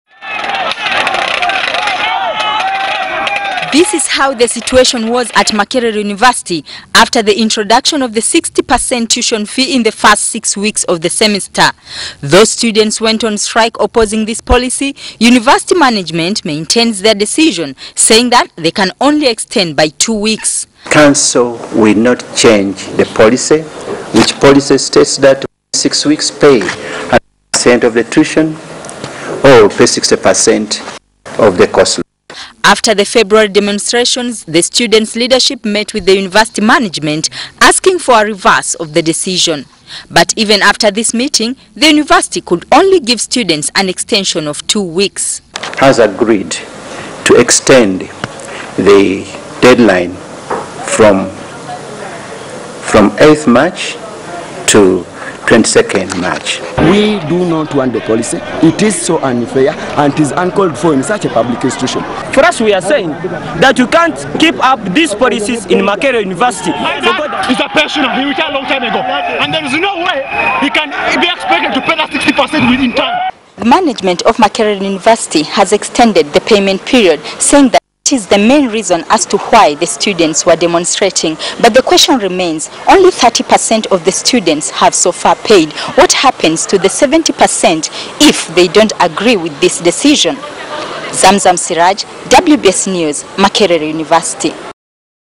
Makerere University vows not change its tuition policy
Makerere University vows not change its tuition policy but only extended the tuition payment schedule from six to eight weeks.

 This follows the students' strikes in February over this policy that they deemed unfair since it required them to pay 60% of their fees.

Zam zam siraji reports